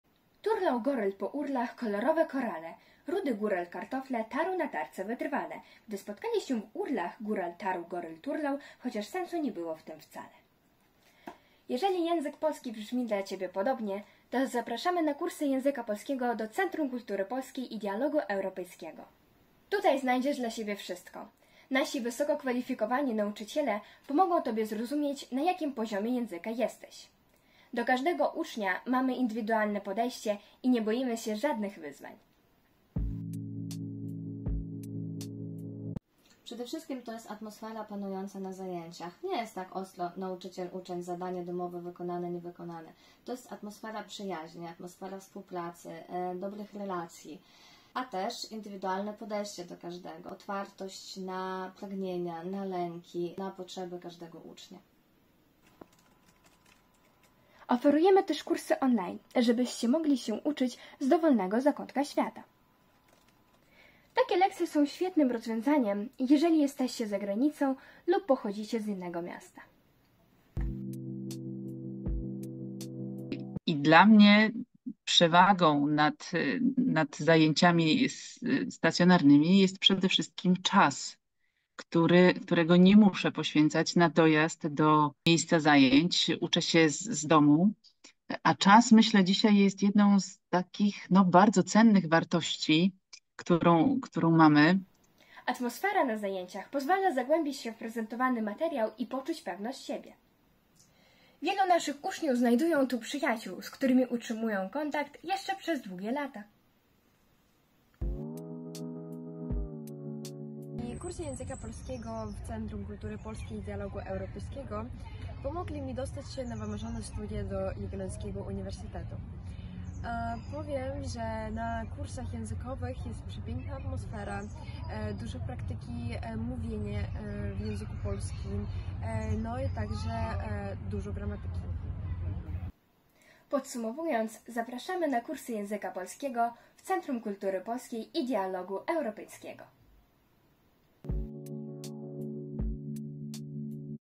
Kursy języka polskiego w Centrum Kultury Polskiej i Dialogu Europejskiego
Centrum Kultury Polskiej i Dialogu Europejskiego w Iwano-Frankiwsku zaprasza na kursy języka polskiego w roku akademickim 2024/2025 dla dzieci, młodzieży i dorosłych.

Program jest skierowany do osób pragnących swobodnie władać językiem polskim dla rozwoju osobistego, podróży, możliwości komunikowania się z partnerami biznesowymi, podjęcia studiów czy pracy w Polsce.

Dofinansowano przez Instytut Rozwoju Języka Polskiego im. Św. Maksymiliana Marii Kolbego ze środków Ministerstwo Edukacji Narodowej. Przedsięwzięcie „Biało-czerwone ABC – utrzymanie placówek oświatowych na Ukrainie cz. IV” realizowane przez Fundację Wolność i Demokracja.